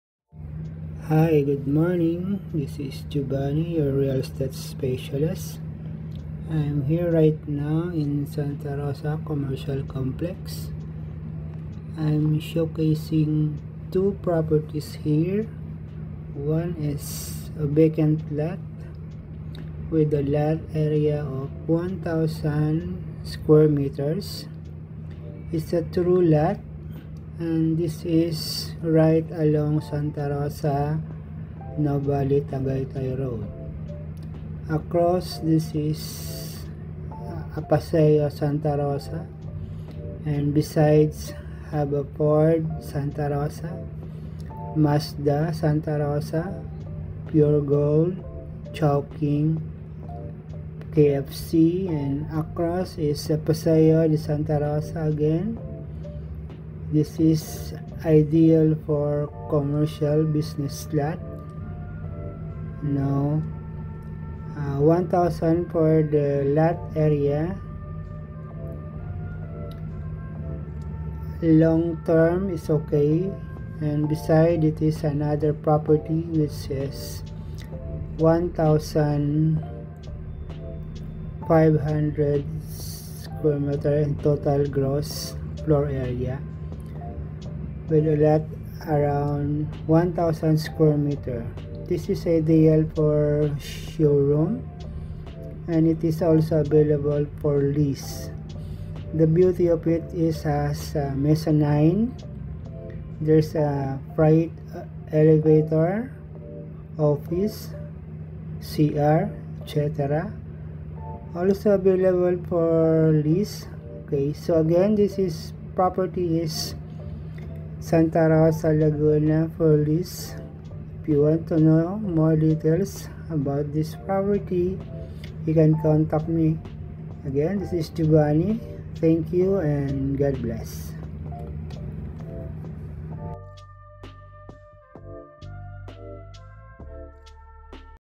Prime Commercial Property in Santa Rosa Commercial Complex Santa Rosa Laguna@jov08TVOffical
2 Commercial Spaces ready for Lease in Santa Rosa Commercial Complex fronting Paseo De Santa Rosa Laguna

✅️1,000 SQM Vacant Lot
✅️1,500 SQM Commercial Space ideal for Showroom

☎️ Call or PM for more info!

Giovanni Espinosa
Real Estate Salesperson
Under PRC 9406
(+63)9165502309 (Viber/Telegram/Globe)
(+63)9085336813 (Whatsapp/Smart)